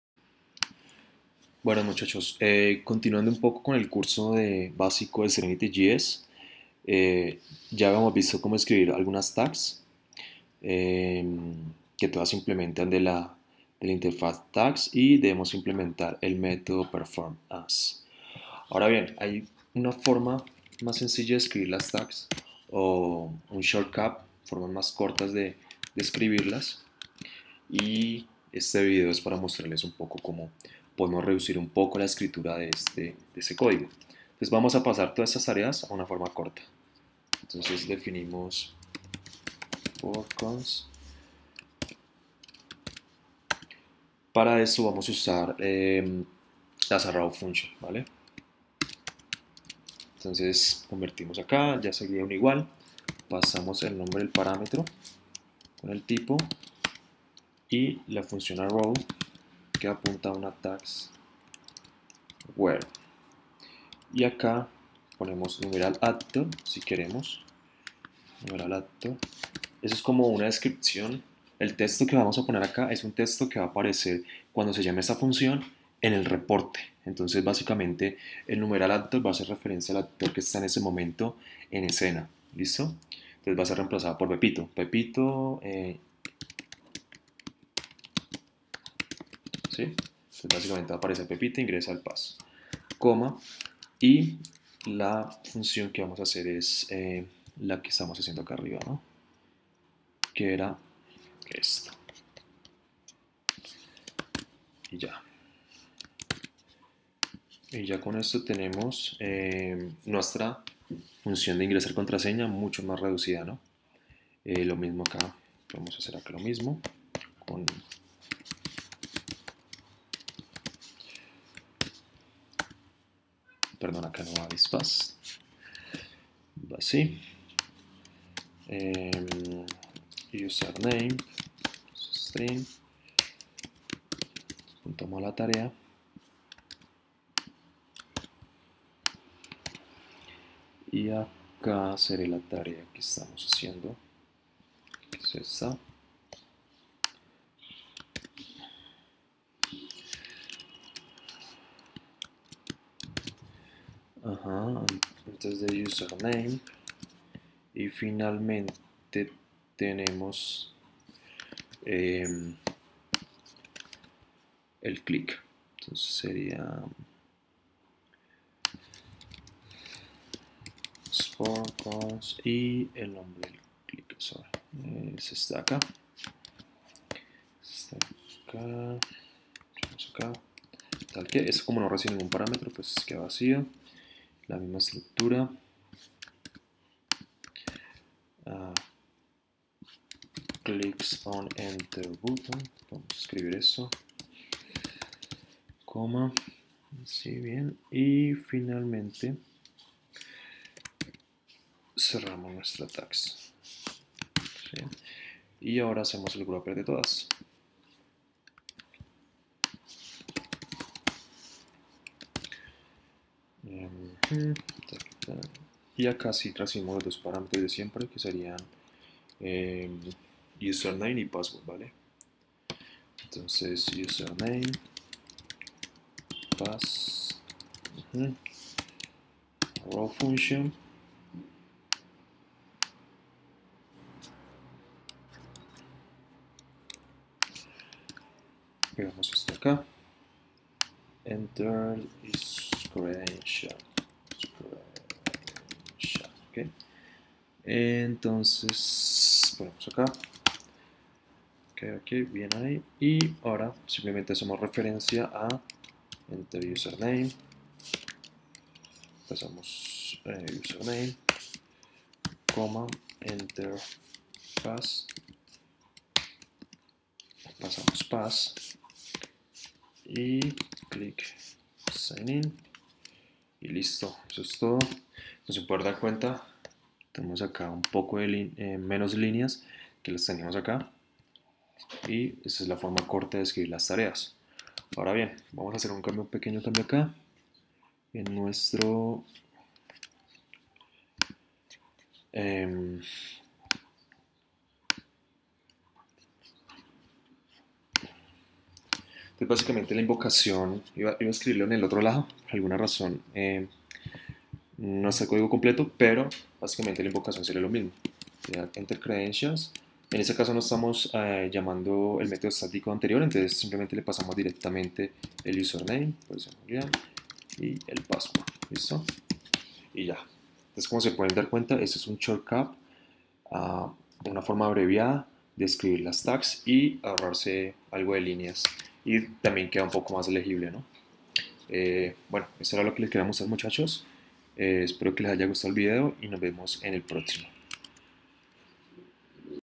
Crear una task de forma corta | Serenity JS | Parte 3
Crear una task en serenity js. Forma corta.
Create serenity js shortcut